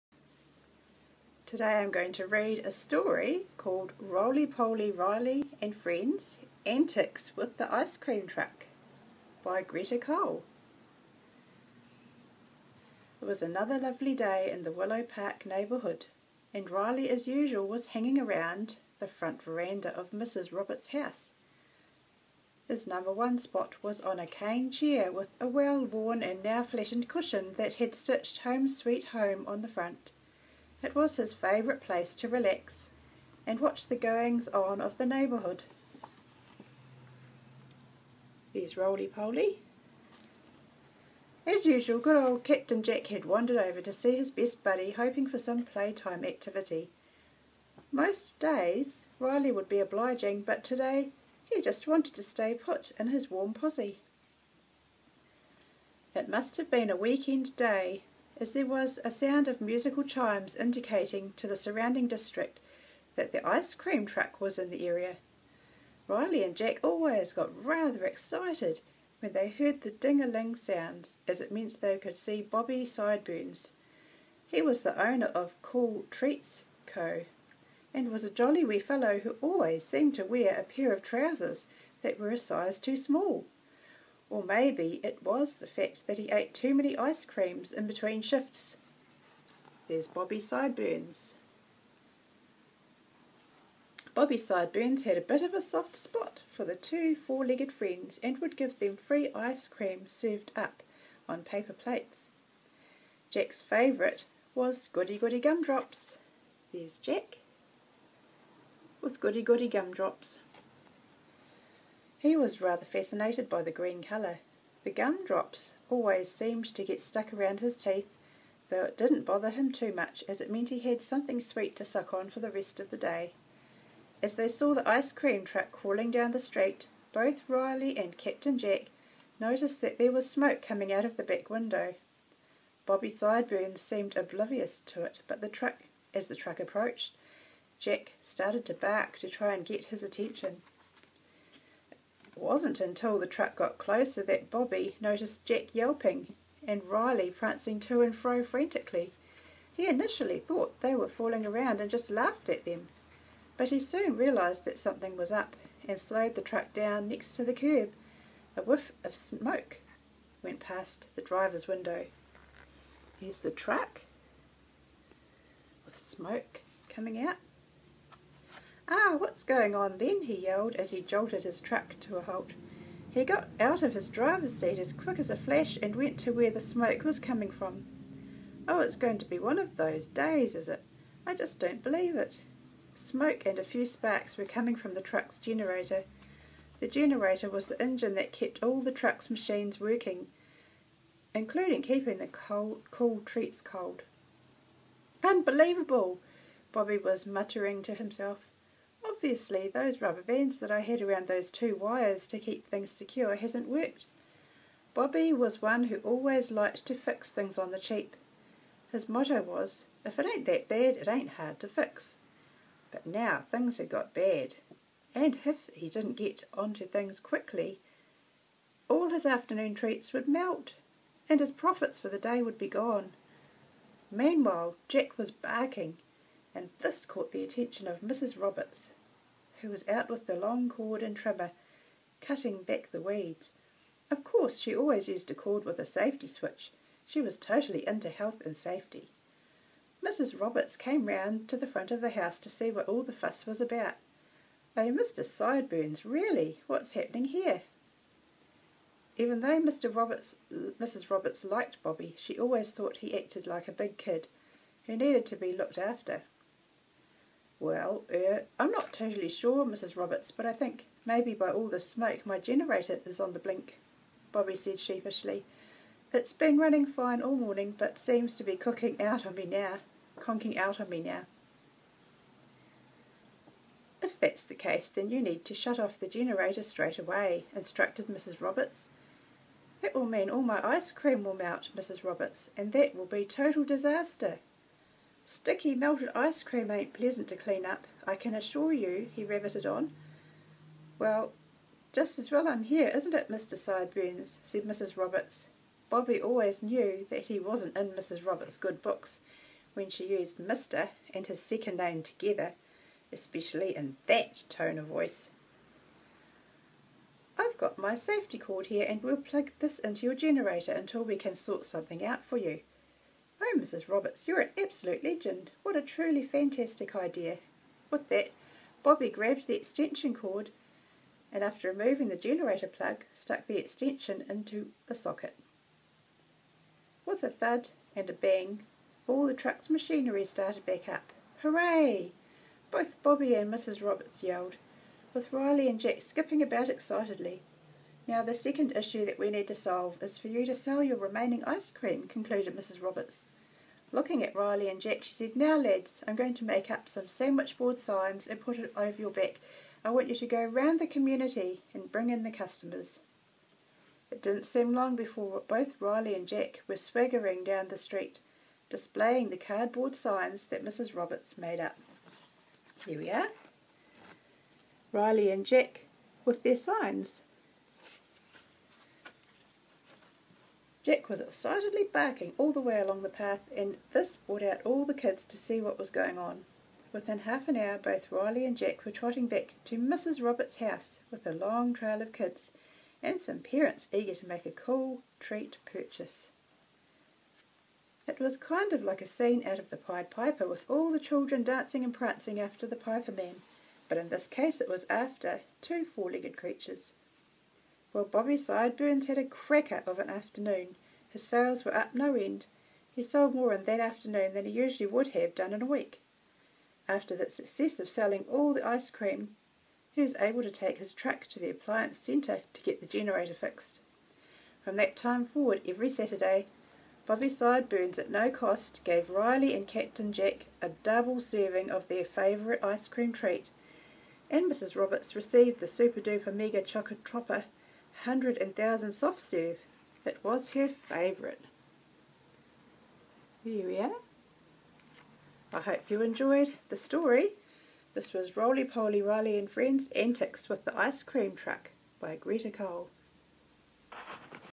Roly Poly Riley and Friends - Antics with the icecream truck - Greta Cole (available now on Amazon)
Roly Poly Riley - antics with the ice cream truck - by Greta Cole, available now on Amazon